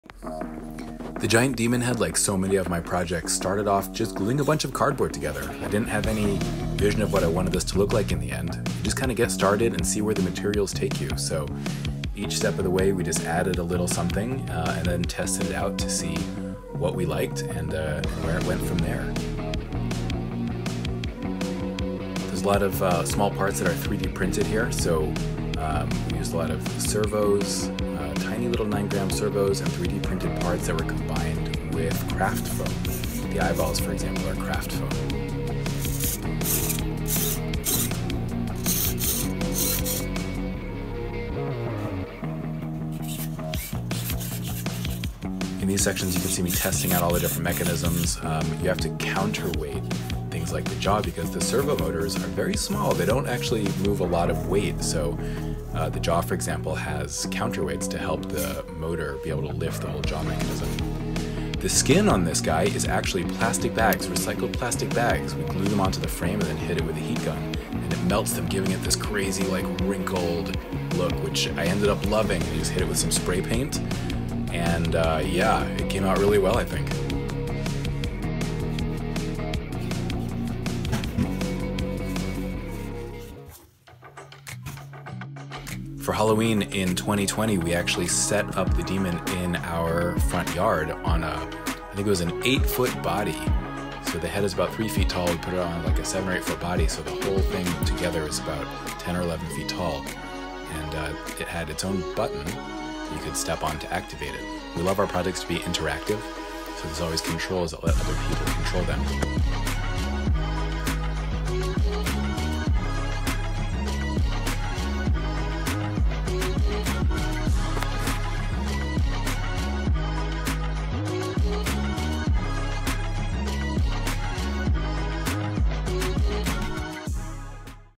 How to make a giant animatronic demon head
This build was the first in what became an annual series of animatronic Halloween creations. Matt Demon is is constructed of cardboard, and skinned with upcycled plastic bags, melted with a hot glue gun. It uses tiny servos, and is controlled by an Arduino.
This and other creations of ours will be displayed at the Arts Center in Carrboro, NC during Summer 2025, so drop by to see them in person!